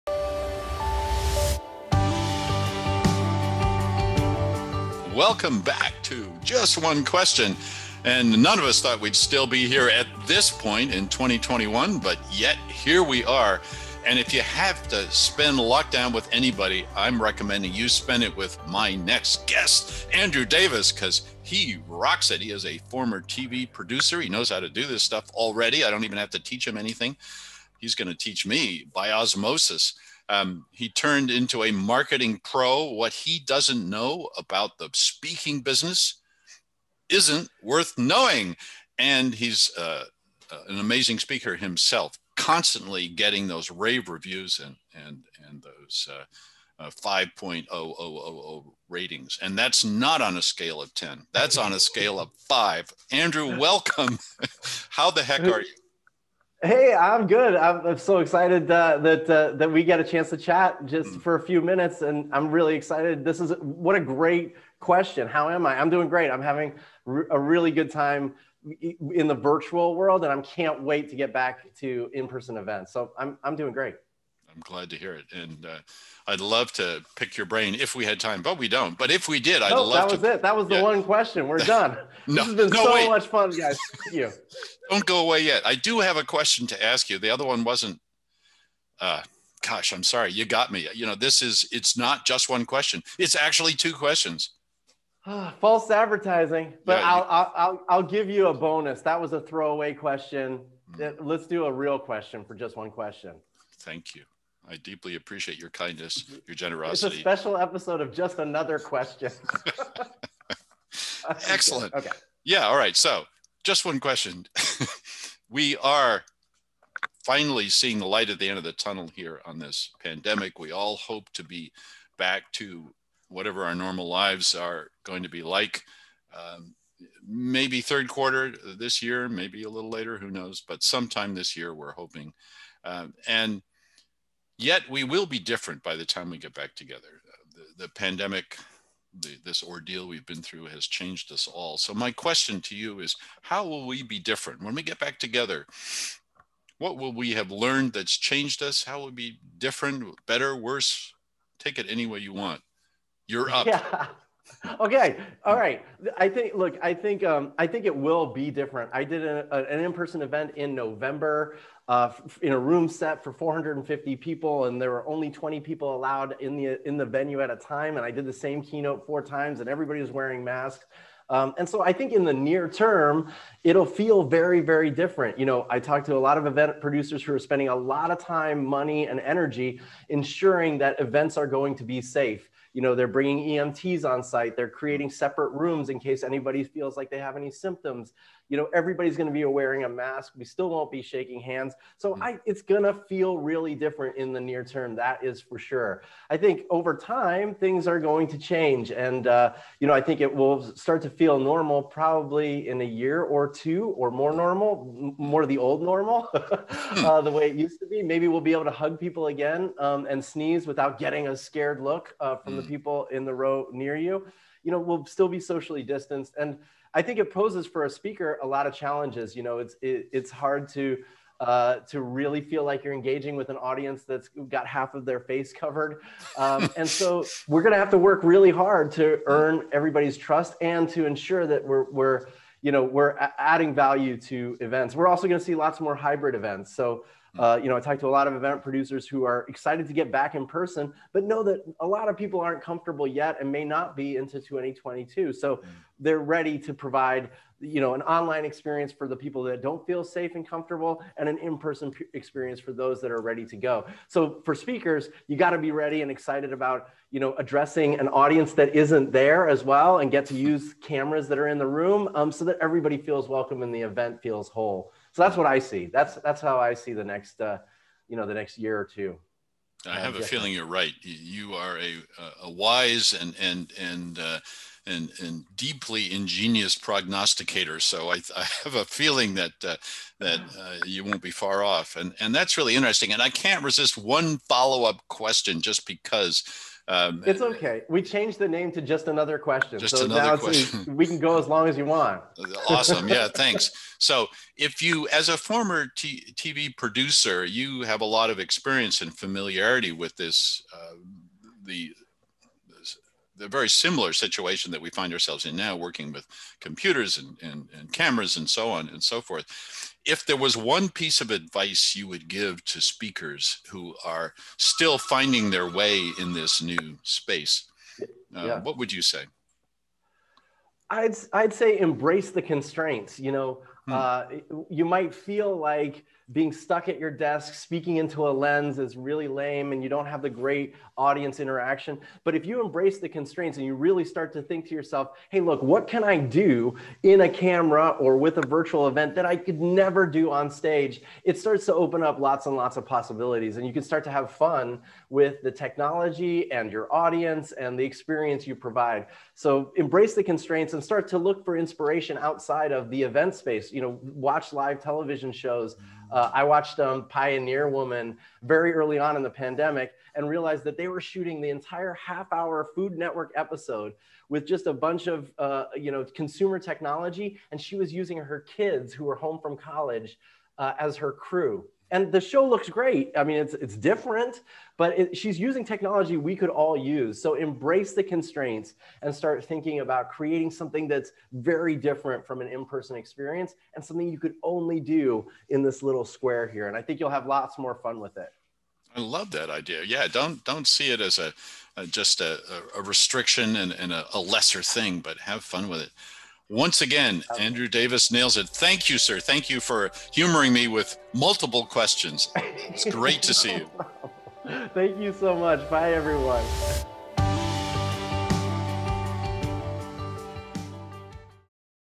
Just One Question Season Episode 46: Andrew Davis former TV producer turned marketing maven.
Andrew Davis is a former TV producer turned marketing maven -- and one of the great speakers on the circuit today who consistently gets knockout ratings.  You can read more about him and check out his sizzle reel